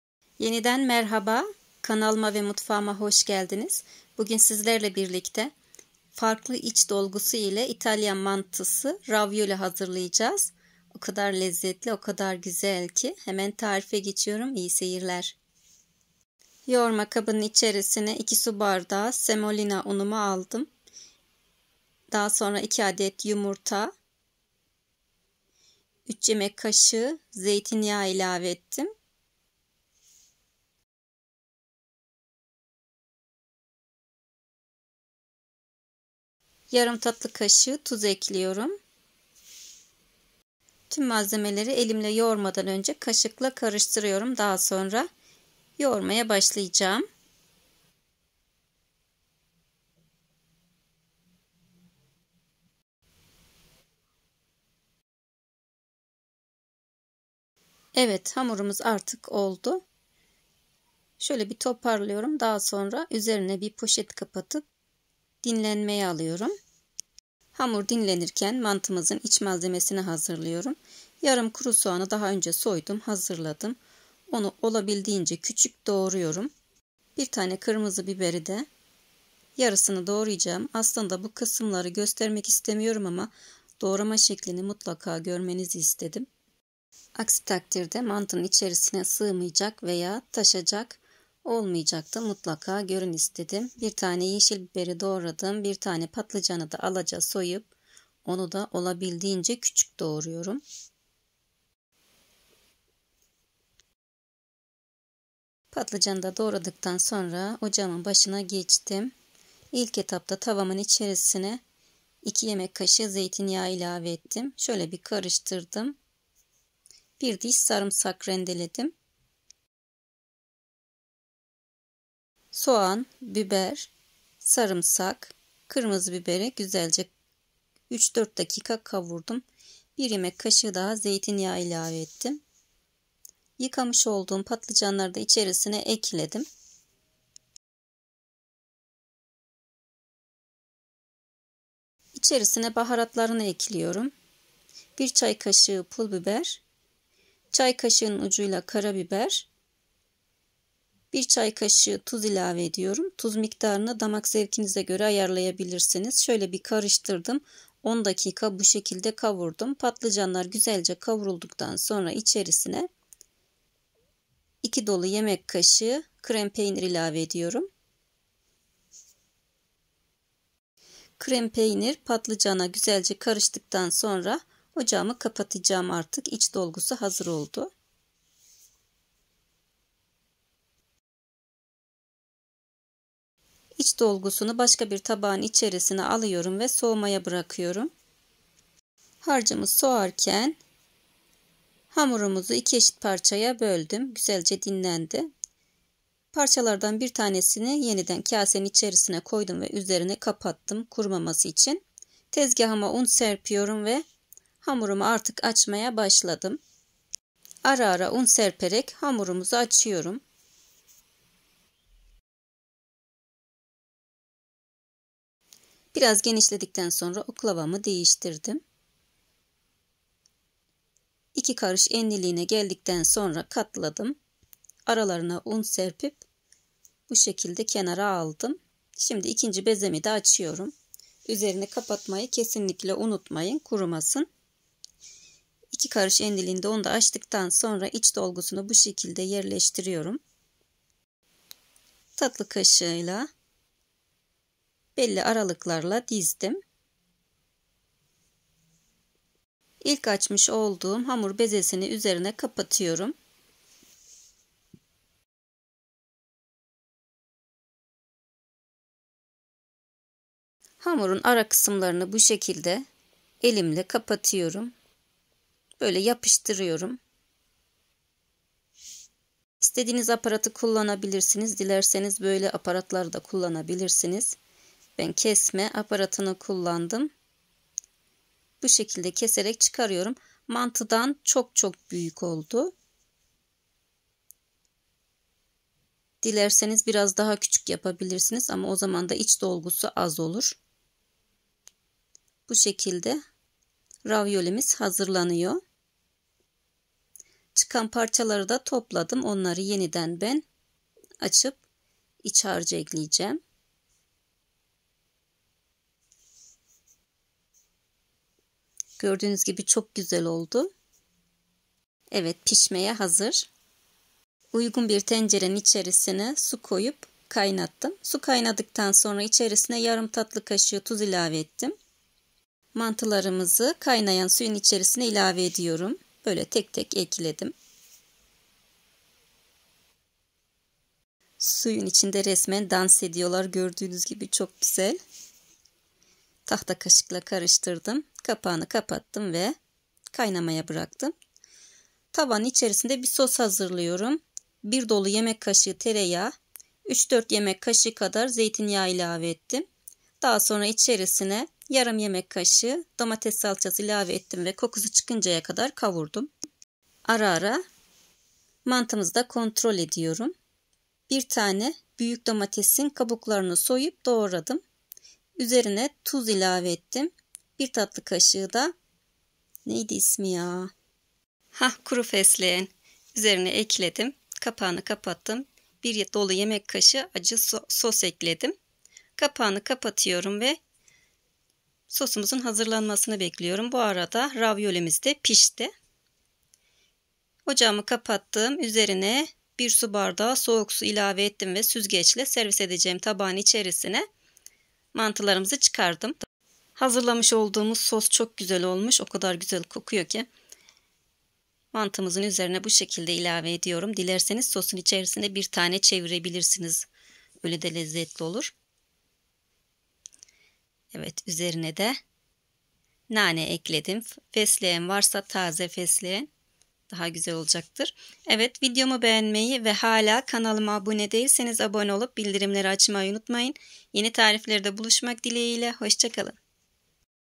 İtalyan Mantısı Ravioli Tarifi 👌 Ravioli Nasıl Yapılır / İtalyan Mantısı Tarifi/ Mantı Tarifi
Kim bu mantıya Hayır diyebilir ki 🥰 italyan makarnası ravioli tarifi

italyan mantısı ravioli tarifi

hamuru için malzemeler
2 adet yumurta
3 yemek kaşığı zeytinyağı
2 su bardağı semolina unu ( un miktarı yumurtanın büyüklüğüne küçüklüğüne göre değişebilir)

ravioli iç dolgusu tarifi
malzemeler
1 iri adet patlıcan (300gr)
yarım kuru soğan (20gr)
1 kırmızı biber (70gr)
1 yeşil biber (45gr)
1 diş sarımsak
3 yemek kaşığı zeytinyağı
2 dolu yemek kaşığı krem peynir
1 çay kaşığı pul biber
1 çay kaşığı tuz

üst sosu için malzemeler

1 dolu yemek kaşığı tereyağ
yarım yemek kaşığı domates salçası
1 büyük domates (270gr)
1 yemek kaşığı acı sos
1 çay kaşığı pul biber
1 çay kaşığı tuz
1 tatlı kaşığı kuru fesleğen